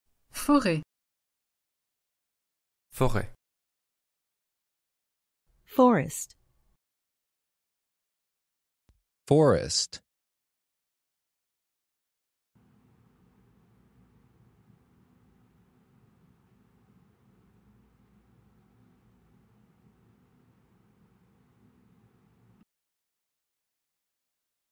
How to say "Forest" in French
Male Voice:
Forêt

Female Voice:
Forêt

Male Voice Translation:
Forest

Female Voice Translation:
Forest